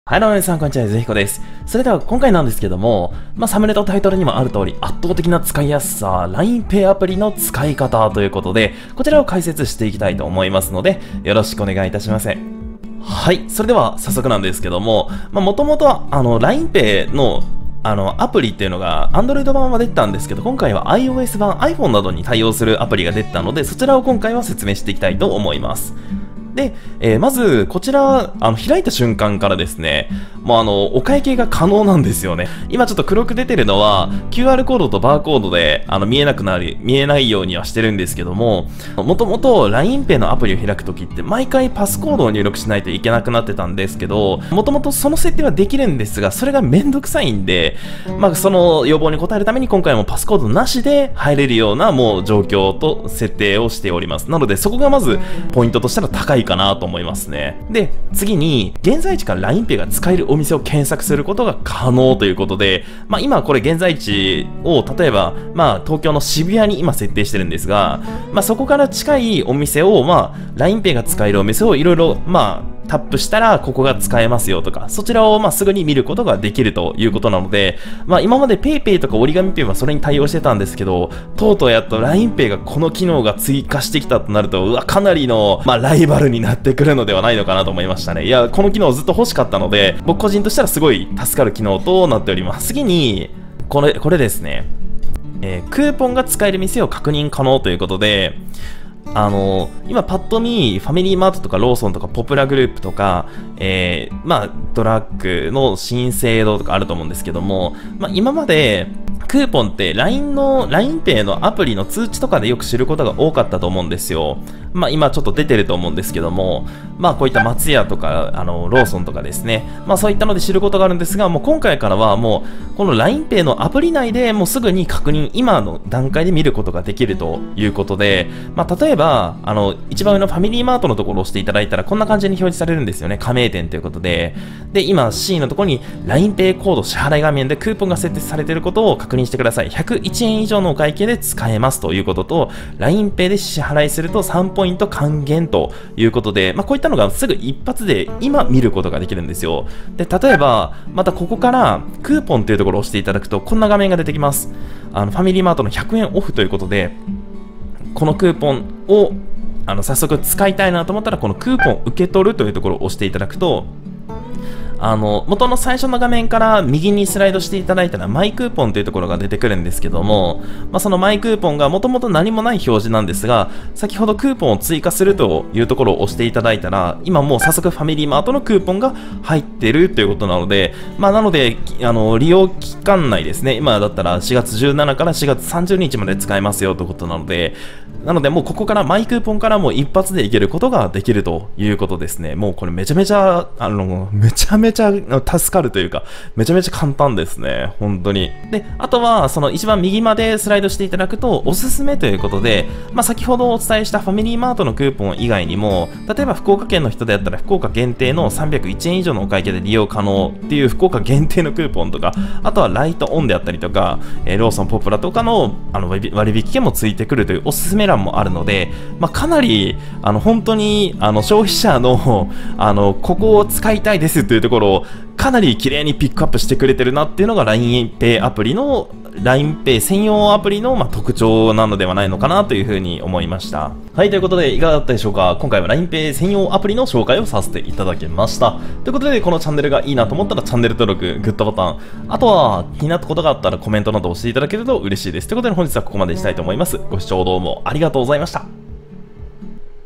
LINEPay専用アプリの使い方！コンビニで即使える【キャッシュレス】
【PR】 想像以上に使いやすいLINEPay専用のアプリ。個人的には待望のマップ機能が追加されたので大変助かっています。今後もLINEPayはさらに強化するような気がするのでかなり期待しています。

主な機能
・画面を開いてすぐにQRコードもしくはバーコード
・マップ機能（利用可能店舗の検索可能）

使用機材
マイク：Rode NT1A
キャプチャーボード：I-O DATA GV-USB3
オーディオインターフェース：YAMAHA AG03